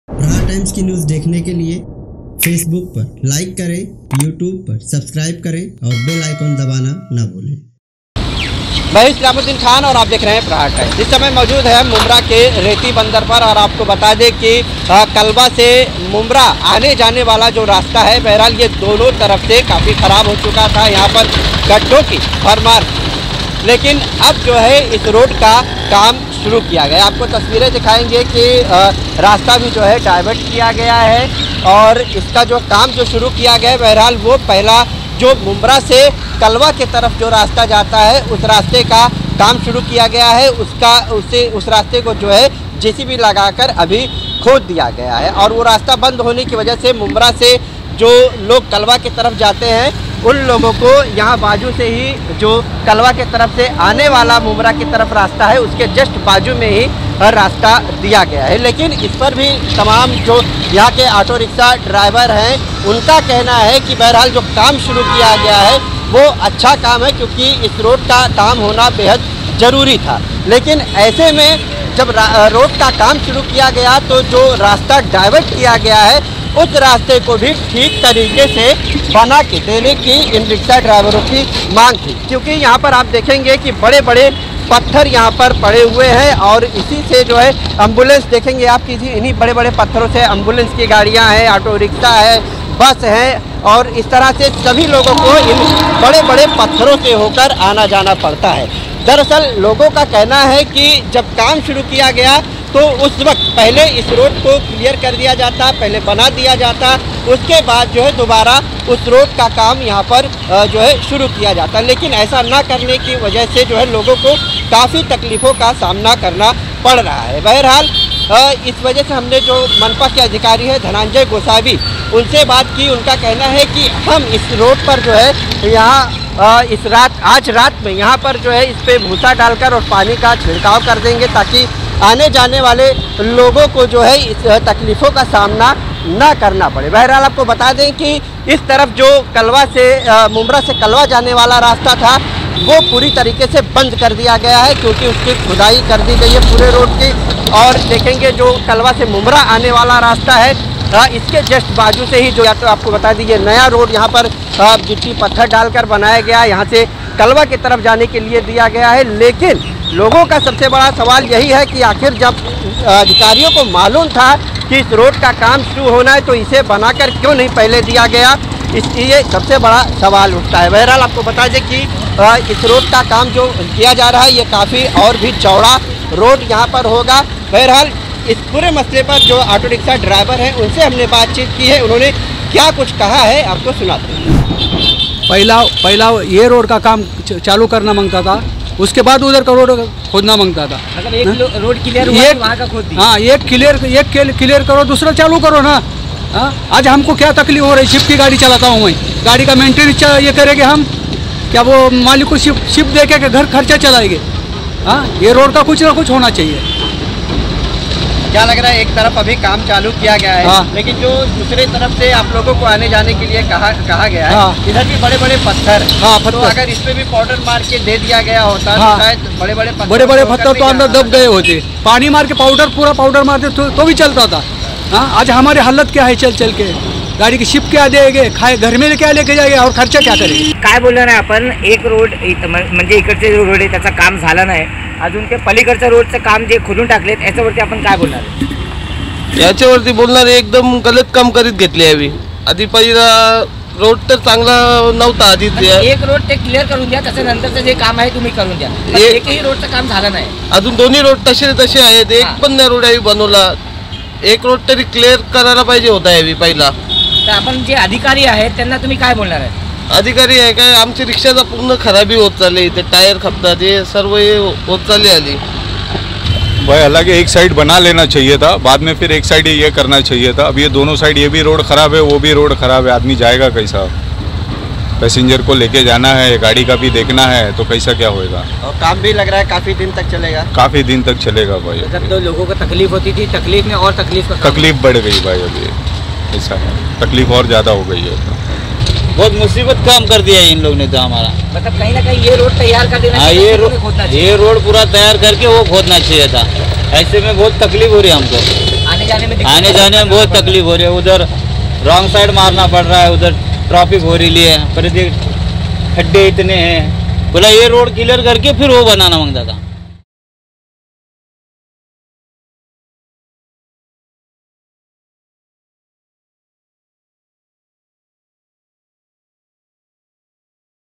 मुम्ब्रा रेतिबन्दर का पत्थरो वाला रोड || Islamuddin Khan || Aapka Prahar Times
रेतिबन्दर रास्ते पर पत्थरों में लोग चलने को मजबूर || Islamuddin Khan

मुंब्रा रेती बंदर रोड का काम शुरू किया गया है मगर ऐसे में मुंब्रा से कलवा जाने वाले रास्ते की खुदाई की गई है मगर रास्ता जिस तरह डिवाइड किया गया है उस रास्ते को बिना बनाकर तैयार किए सिर पत्थर डालकर छोड़ दिया गया है जिस पर लोगों को आने-जाने में तकलीफ हो रही है